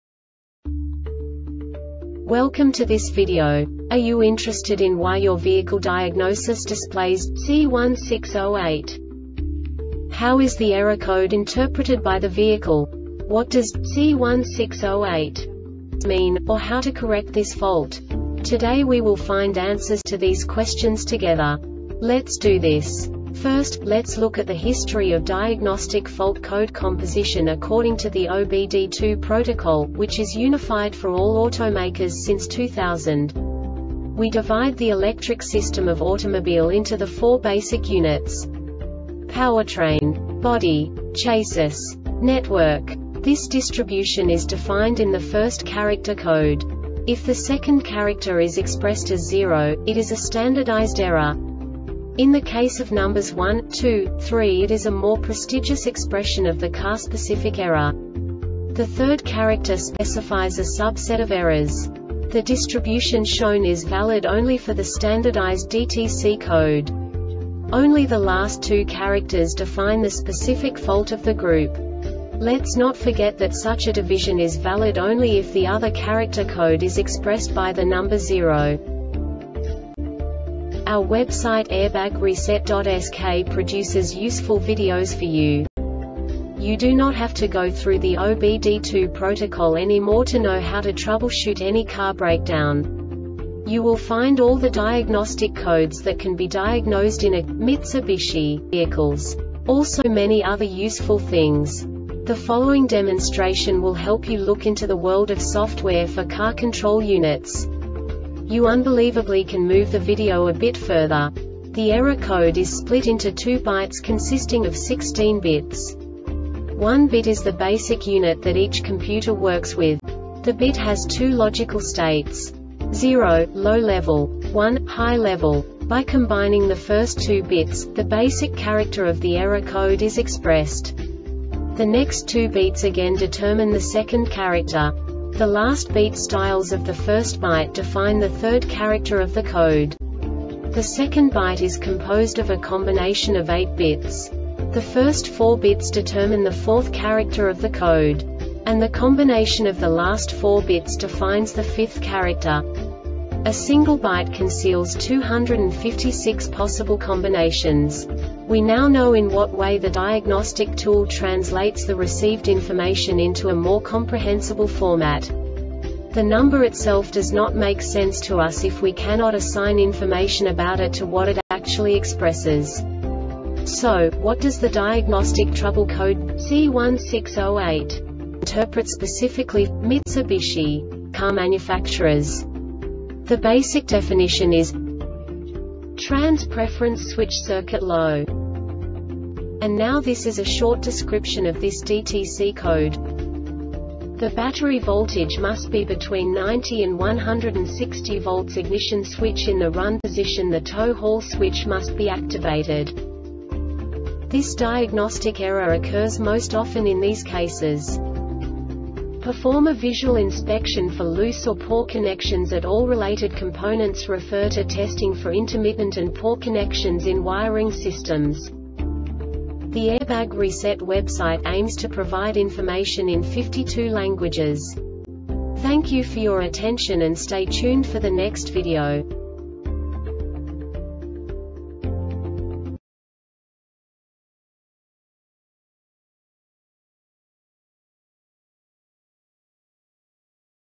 DTC Mitsubishi C1608 Short Explanation
Contents:
0:21 Basic DTC analysis according to OBD2 protocol standard.
1:48 Insight into programming
2:58 A diagnostically understandable description of the Diagnostic Trouble Code
3:05 Basic error definition

Make:
Mitsubishi
Code:
C1608
Definition:
Trans Preference Switch Circuit Low
Description:
The battery voltage must be between 9.0 and 16.0 volts.Ignition switch in the RUN position.The Tow or Haul switch must be activated.
Cause:
Perform a visual inspection for loose or poor connections at all related components.Refer to Testing for Intermittent and Poor Connections in Wiring Systems.